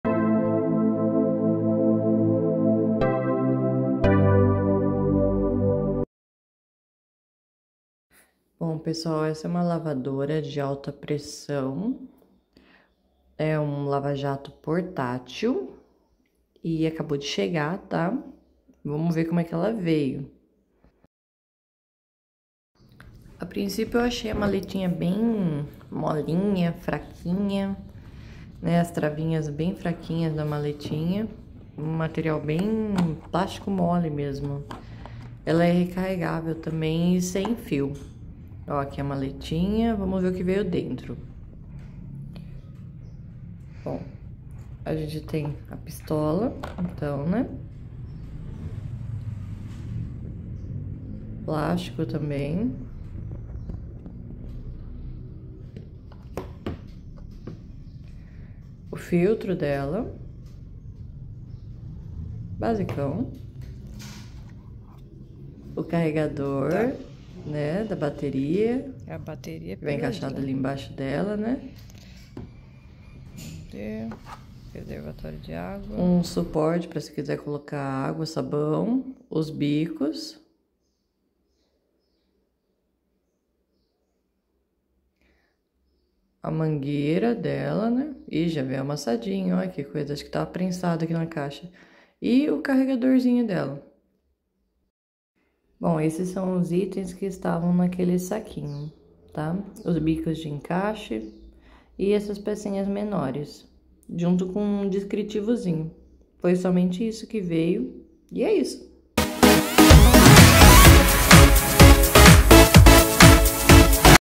Unboxing da Lavadora Alta Pressão Lava Jato Portátil Recarregável S/fio
Apenas um unboxing desta Lavadora portátil que compramos.